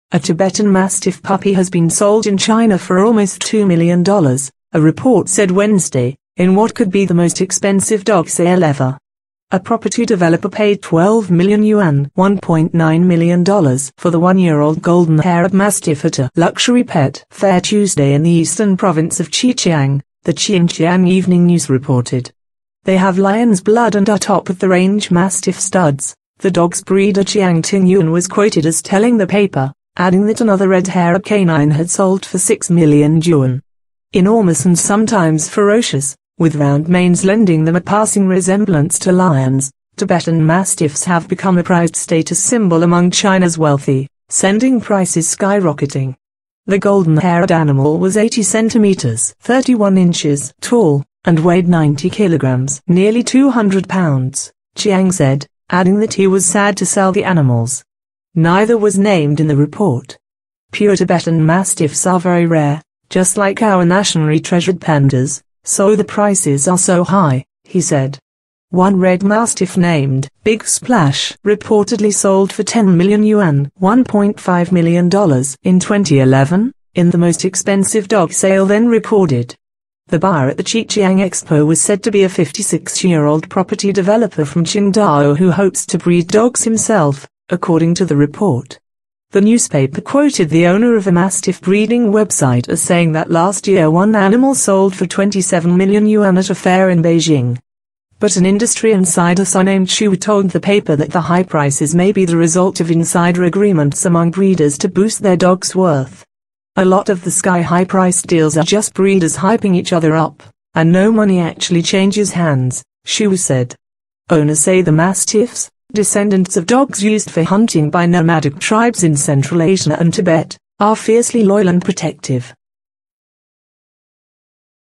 Tibetan Mastiff Dog Sold For $2 million In China
A Tibetan mastiff puppy has been sold in China for almost $2 million, a report said Wednesday, in what could be the most expensive dog sale ever. A property developer paid 12 million yuan ($1.9 million) for the one-year-old golden-haired mastiff at a "luxury pet" fair Tuesday in the eastern province of Zhejiang, the Qianjiang Evening News reported. "They have lion's blood and are top-of-the-range mastiff studs," the dog's breeder Zhang Gengyun was quoted as telling the paper, adding that another red-haired canine had sold for 6 million yuan. Enormous and sometimes ferocious, with round manes lending them a passing resemblance to lions, Tibetan mastiffs have become a prized status symbol among China's wealthy, sending prices skyrocketing. The golden-haired animal was 80 centimetres (31 inches) tall, and weighed 90 kilograms (nearly 200 pounds), Zhang said, adding that he was sad to sell the animals. Neither was named in the report. "Pure Tibetan mastiffs are very rare, just like our nationally treasured pandas, so the prices are so high," he said. One red mastiff named "Big Splash" reportedly sold for 10 million yuan ($1.5 million) in 2011, in the most expensive dog sale then recorded. The buyer at the Zhejiang expo was said to be a 56-year-old property developer from Qingdao who hopes to breed dogs himself, according to the report. The newspaper quoted the owner of a mastiff breeding website as saying that last year one animal sold for 27 million yuan at a fair in Beijing. But an industry insider surnamed Xu told the paper that the high prices may be the result of insider agreements among breeders to boost their dogs' worth. "A lot of the sky-high priced deals are just breeders hyping each other up, and no money actually changes hands," Xu said. Owners say the mastiffs, descendants of dogs used for hunting by nomadic tribes in central Asia and Tibet, are fiercely loyal and protective.